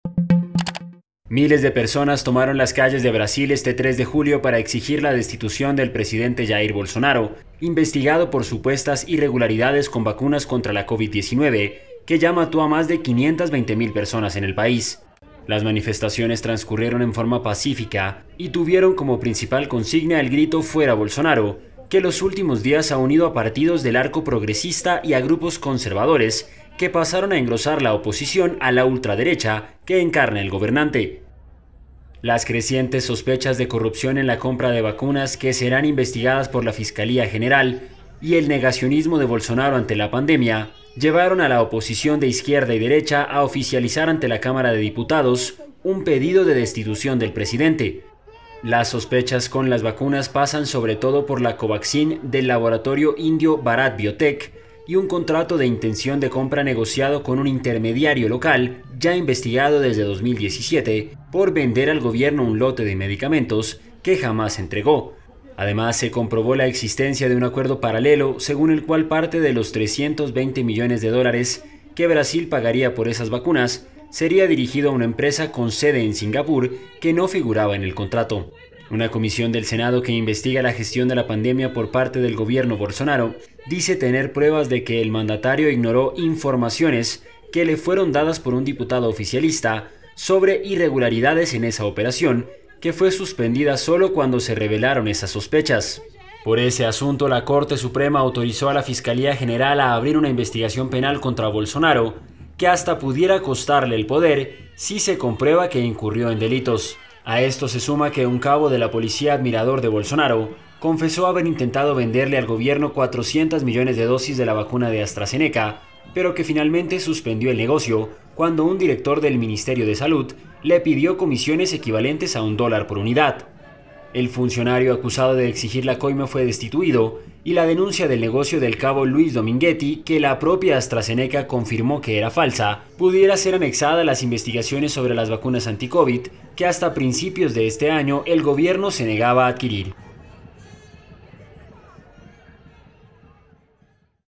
Brasileños gritan "Fuera Bolsonaro" tras el escándalo de las vacunas
Miles de personas tomaron las calles de Brasil este sábado para exigir la destitución del presidente Jair Bolsonaro, investigado por supuestas irregularidades con vacunas contra la covid-19, que ya mató a más de 520.000 personas en el país.

¡Visítenos!